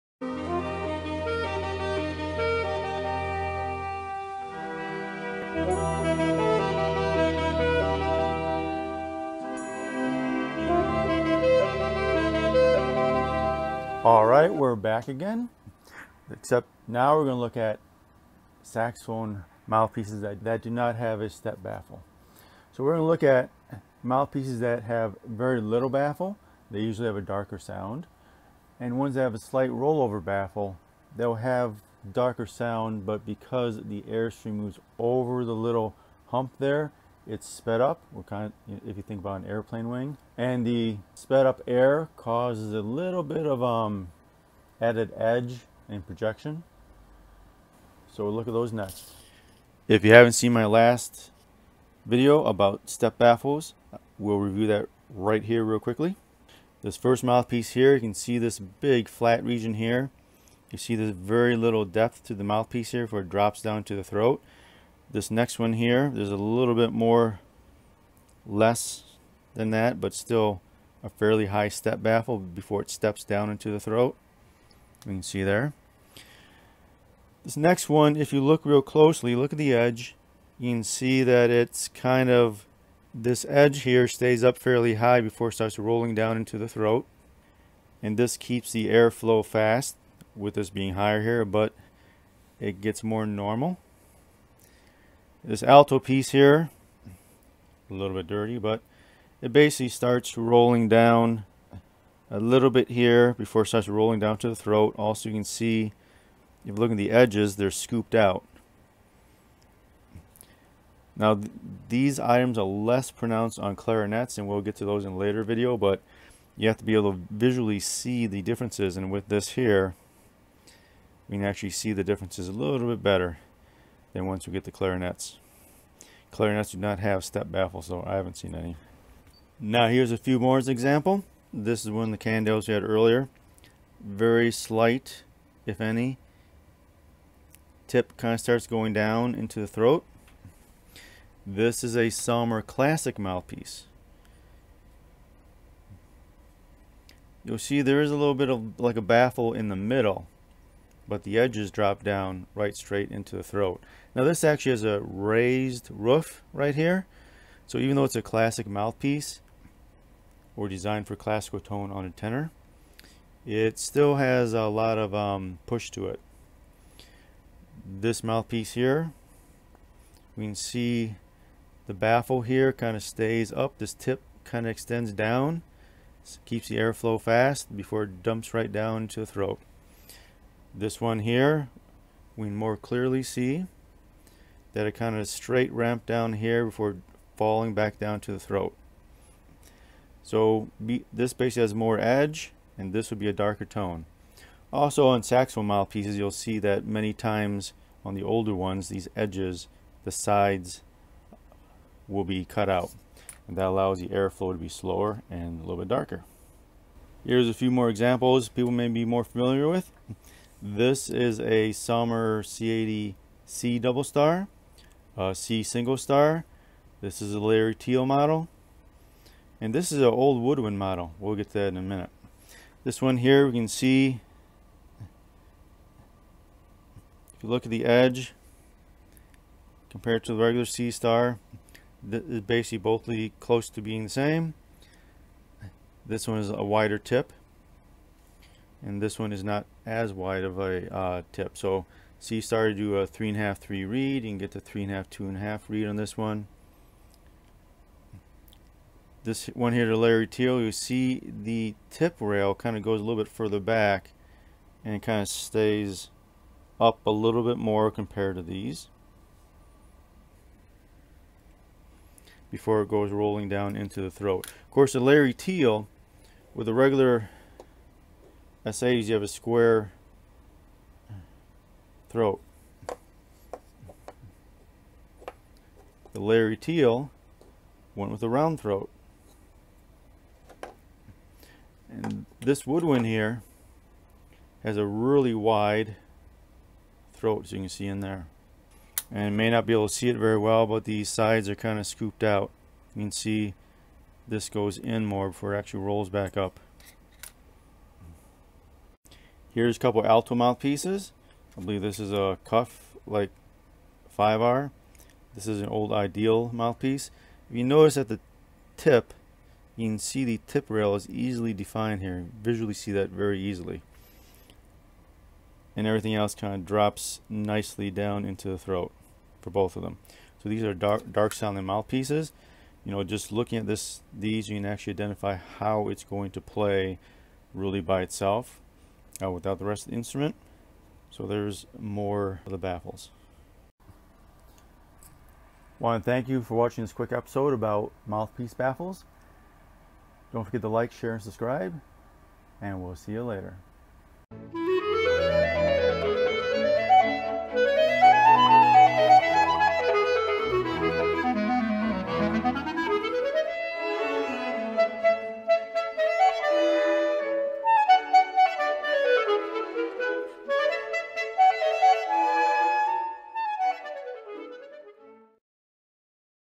Saxophone Clarinet Mouthpiece Flat and Roll-Over Baffles
Saxophone Clarinet Mouthpieces Flat and Roll-Over Baffles.  Learn to identify the differences.